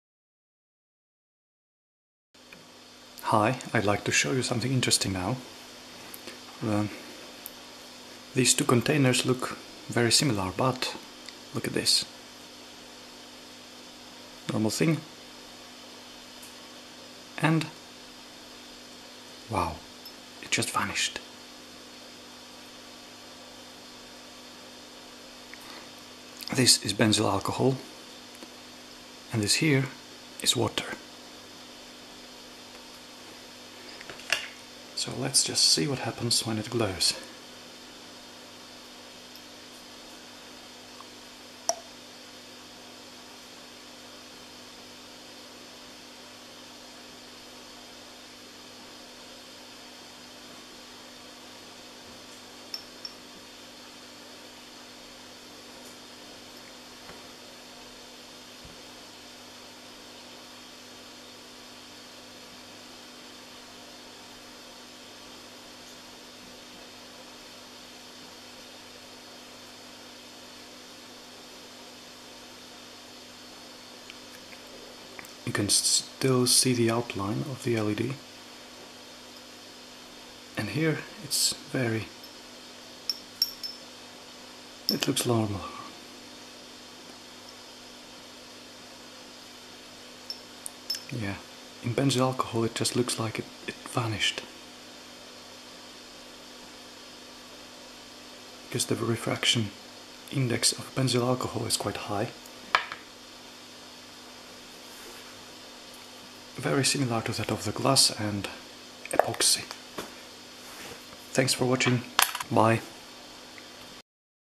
Water and benzyl alcohol
LED submerged in water and benzyl alcohol.

I'll make a better video some time.